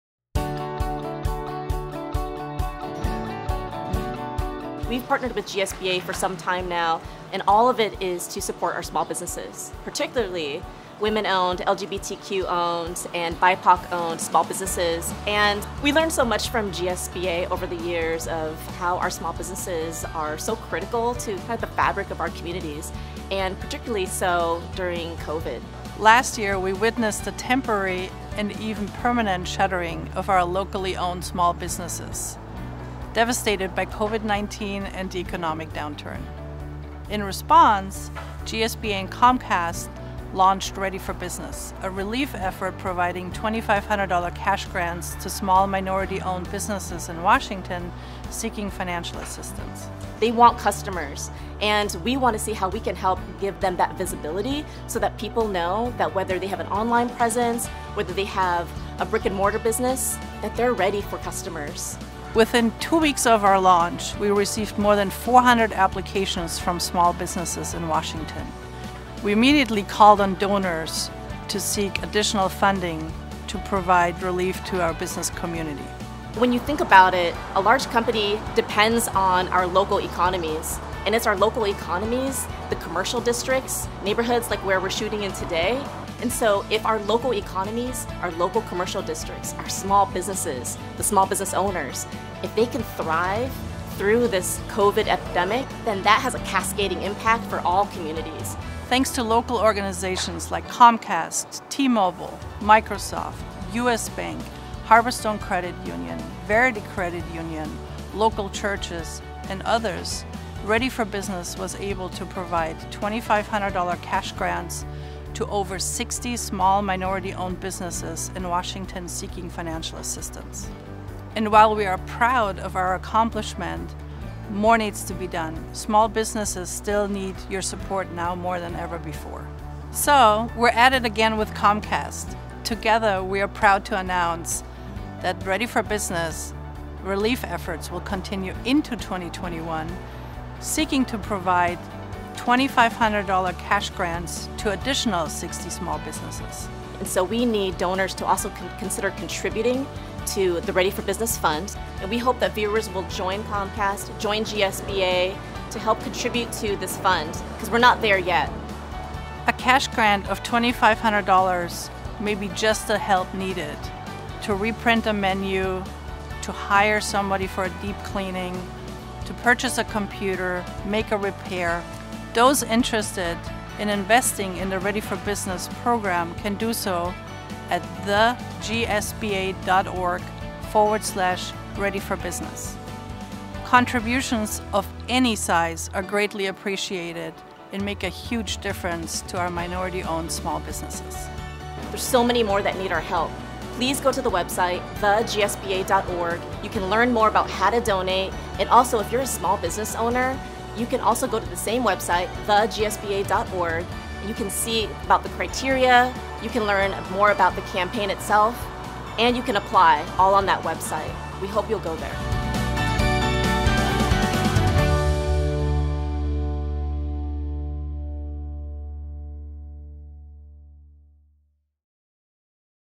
“Ready For Business” Small Business Relief Fund Expands Statewide
GSBA – Washington’s LGBTQ and allied chamber of commerce – and Comcast Washington are now accepting applications from small businesses throughout the state seeking financial assistance. Eligible BIPOC, LGBTQ, and women-owned small businesses may apply March 15 through Friday, April 9, 2021 at  theGSBA.org/ready-for-business. Selected businesses will receive $2,500 grants from the Ready for Business Fund formed by GSBA and Comcast.

The Ready for Business Fund has expanded to support small businesses statewide this year with a $50,000 investment from Comcast and GSBA continuing its role as the fund and program manager. The statewide expansion will also focus on providing support for qualifying small businesses located in rural areas of Washington who have been particularly impacted by the pandemic and lack of proximity to resources.

The grant application is short and easy to complete. Submissions will be reviewed by a committee of volunteers from around the state representing diverse backgrounds and skill sets. All grant recipients will receive wrap-around services from GSBA that include membership and business consulting. All grant applicants will be notified the week of April 26.